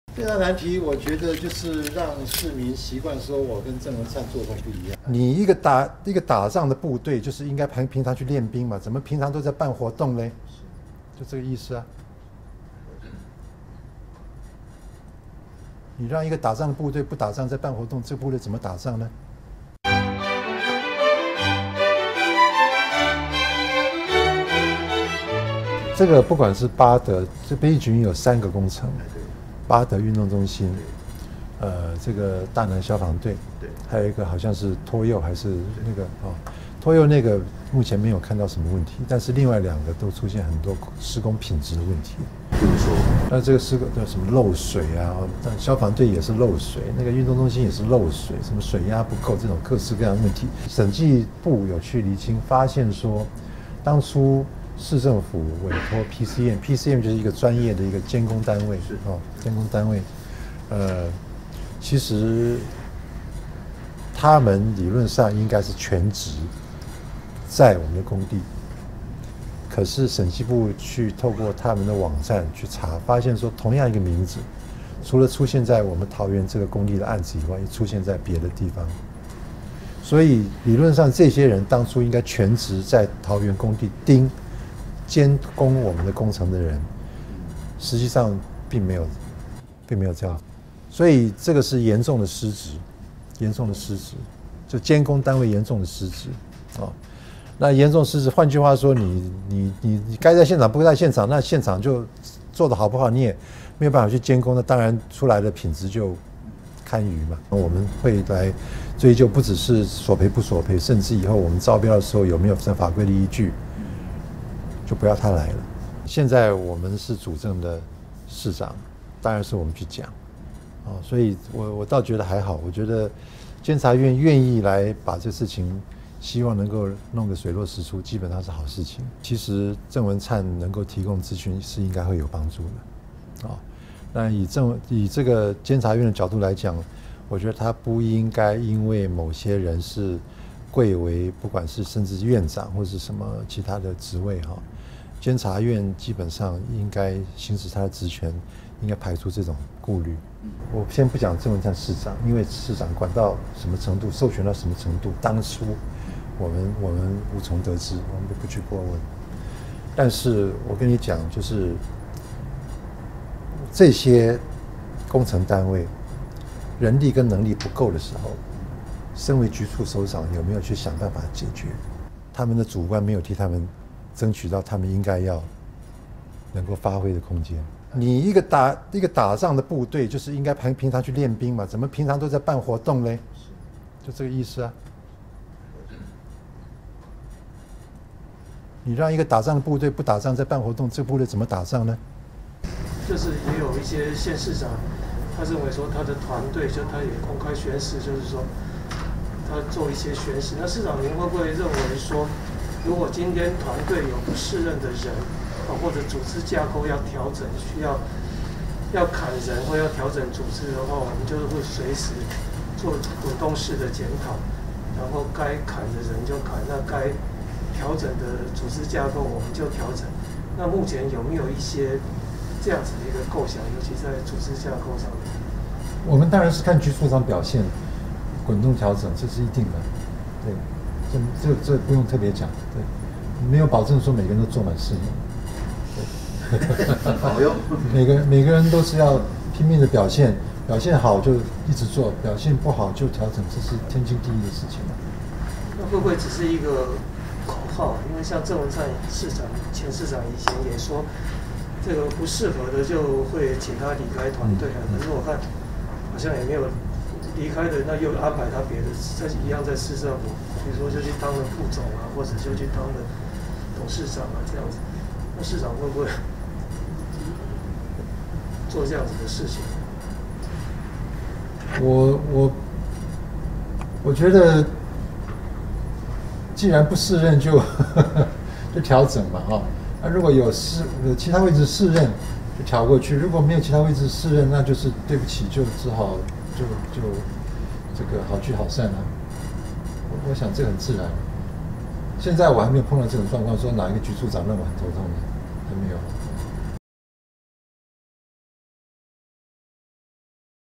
百里侯百日新政專訪 張善政批前朝狂辦活動
九合一大選落幕，全台縣市長去年十二月廿五日上任至今即將滿一百天，聯合報系從今天起推出「百里侯百日新政」系列報導，以獨家專訪、內幕報導、深度解析等角度，為讀者體檢百里侯的施政方針。
桃園市長張善政就職將滿一百天，外界關心「桃園大祕寶」公共工程體檢進度。隸屬「北景雲計畫」的桃園八德運動中心曾發生羽球場天花板崩塌事件，張善政接受本報專訪時首度表示，下周五他接受監委約詢時將說明北景雲計畫缺失，被通報列管高達二二六項，桃園眾多工程缺失來自前市府內控失能。
張善政也指出，前市長鄭文燦縱容局處首長，養成愛辦活動博取掌聲的官場文化，施工期間PCM（委託工程專業管理）有四名專任管理人員和土建人員違約兼職，監工不確實。但鄭文燦昨晚截稿前未回應。
張善政表示，站在公務員角度出發，相關主管沒有替基層爭取到可發揮專業的空間，卻愛辦活動博取掌聲，「這種局長真的該辦」；先不管鄭文燦是否有缺失，當初如何授權，「這樣的縱容，也該負責」。前市府動不動就動工的文化，只是冰山一角，一個部隊應該要練兵，如果都在辦活動，怎麼打仗？
北景雲計畫包括八德國民運動中心、大湳消防分隊廳舍和大湳社福館三項工程，八德運動中心羽球場在去年九合一大選期間天花板崩落，驚悚畫面登上國際媒體；北景雲遭檢討，監院卻在選後發函約詢張善政，外界質疑「搞錯對象」。
張善政表示，站在監察院角度，如果北景雲統包工程缺失全貌要更完整，鄭文燦提供的資訊會有幫助，監院想要完整資訊就應行使職權，「不該因對方是行政院副院長而有所顧慮」；若鄭文燦和相關人員主動前往說明，也是做法之一。